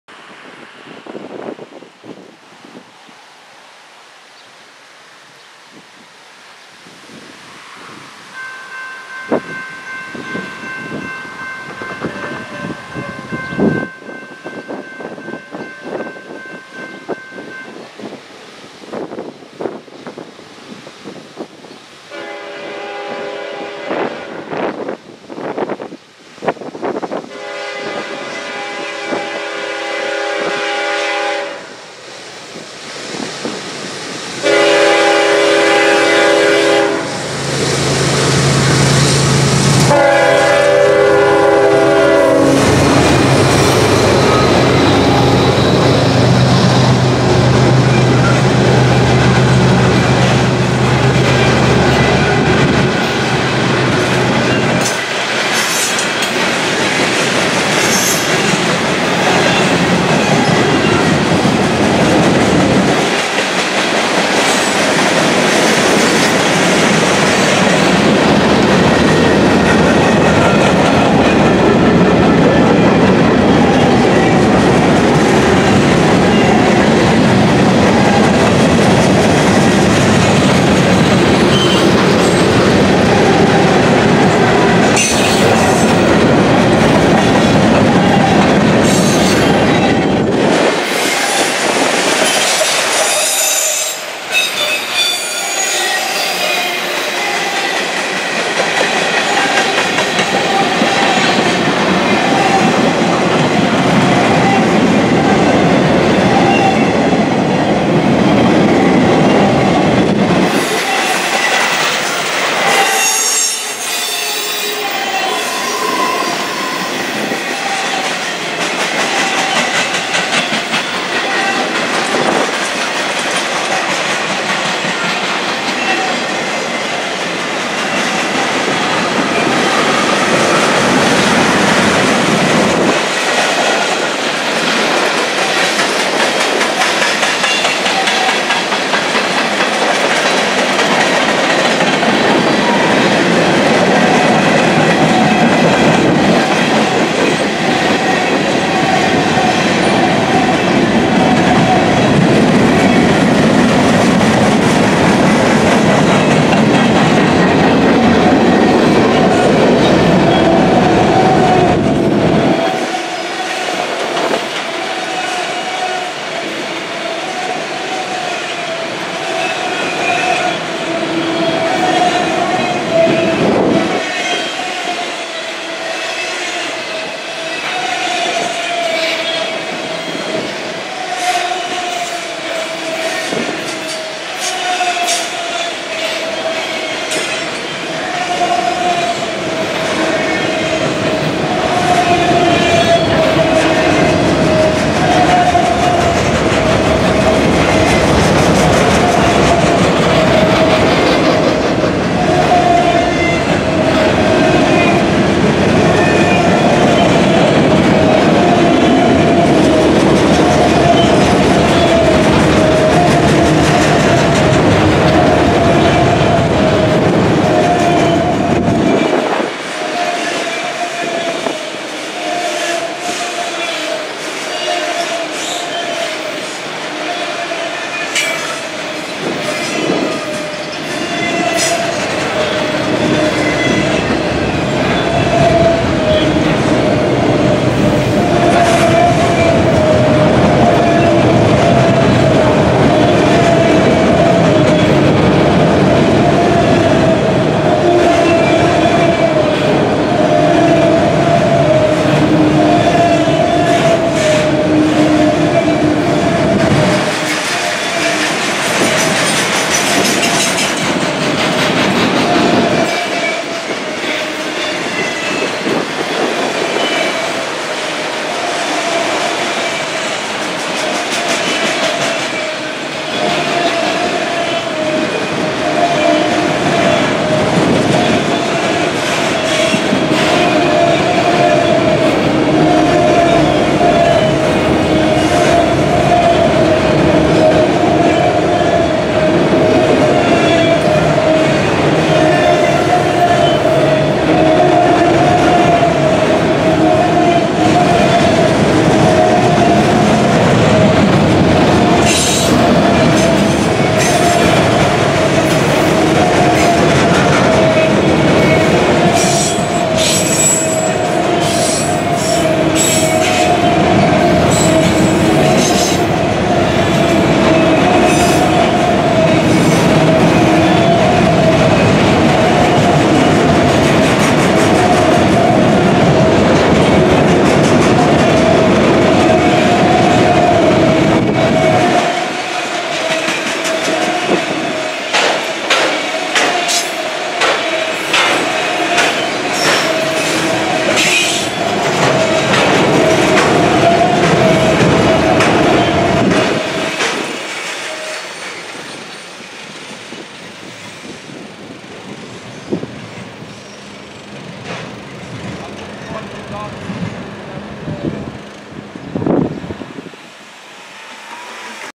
NORFOLK SOUTHERN GE ES40DC GEVO Southbound Mix Freight
A southbound Manifest Mix Freight Lead by Norfolk Southern GE ES40DC #7689 Engine passing through at Milepost Rosemont.